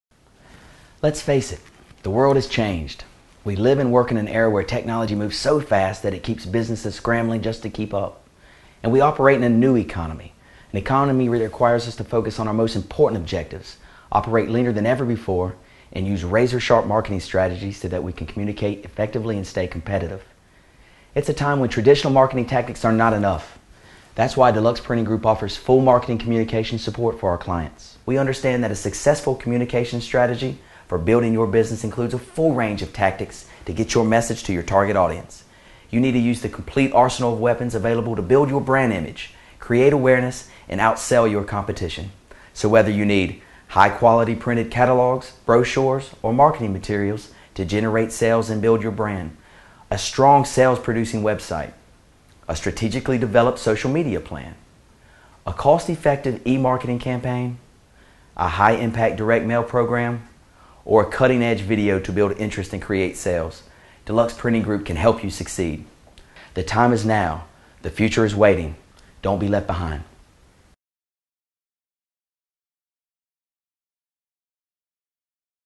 Deluxe Printing Group - The Range Of Services We Offer
Deluxe Printing Group President Chad East discusses the new creative services that Deluxe Printing Group can now offer their clients. Services including: Strategic Planning, Copywriting, E-Marketing, Photography, Videography, Scripting, Graphic Design, as well as Printing and Direct Mail capabilities. Deluxe Printing Group is now a comprehensive, one stop partner for businesses seeking marketing services of any kind.